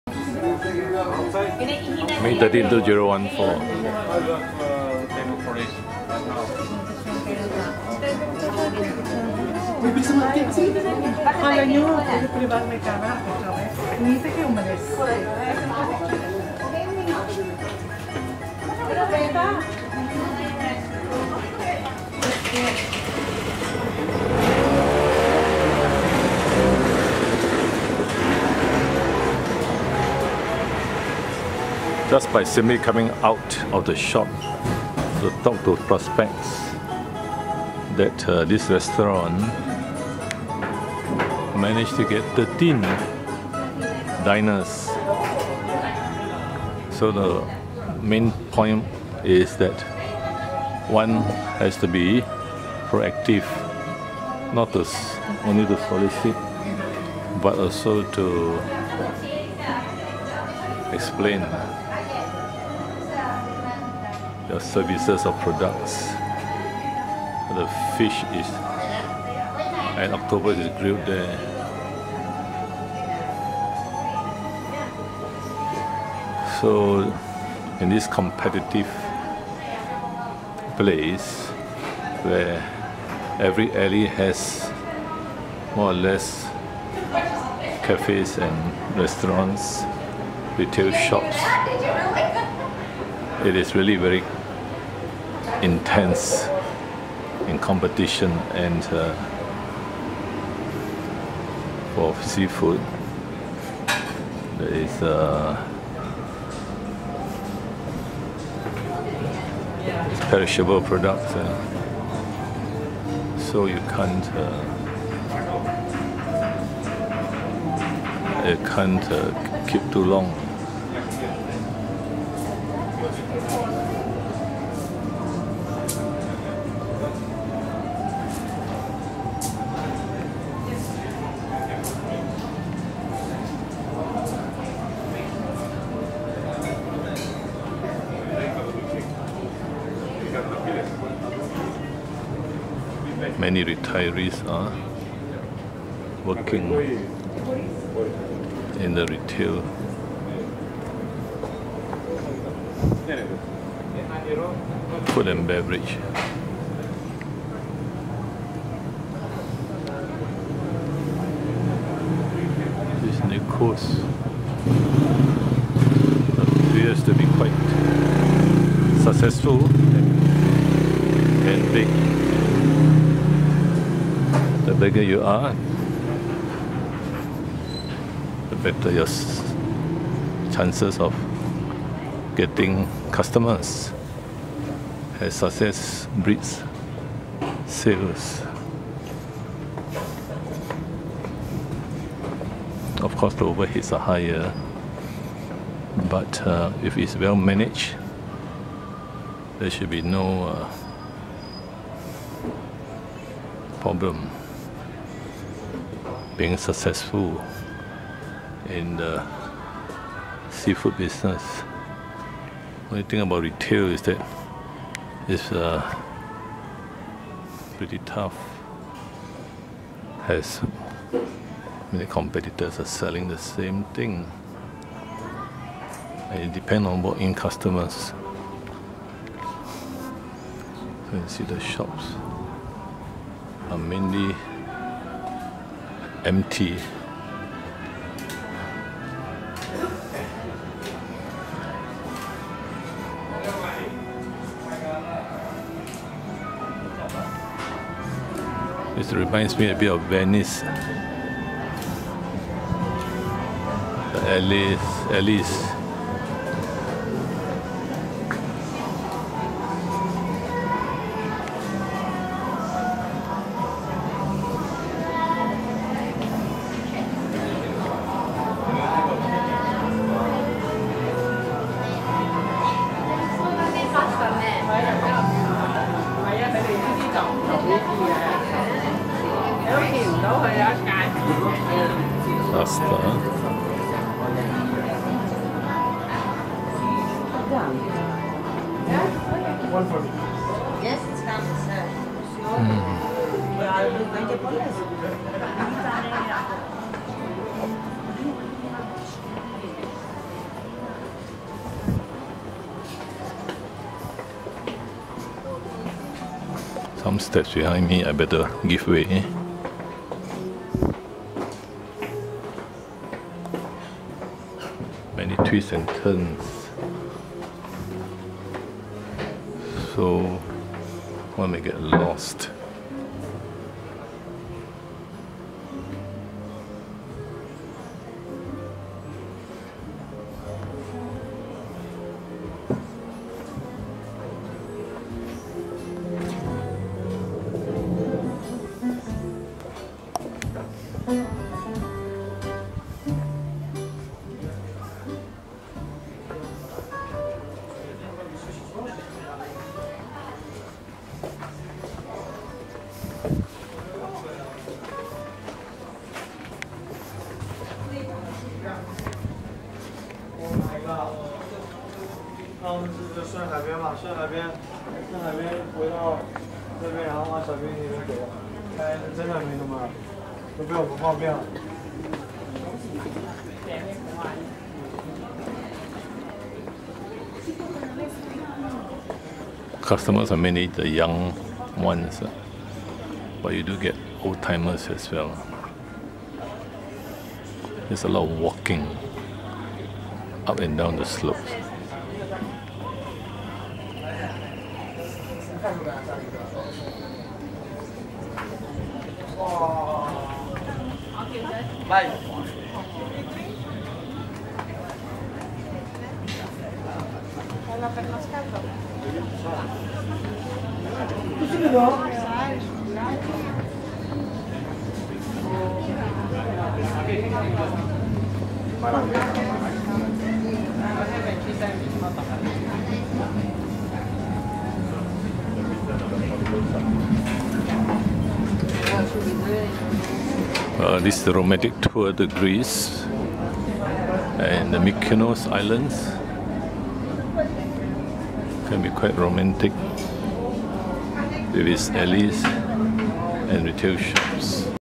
Mykonos shophouses and alley ways
Intense competition with the same seafood being sold. The restaurant staff must be proactive to solicit for clients in this low tourist season as seafood is perishable. In this case, 13 Filipino tourists were enticed to try the sea food.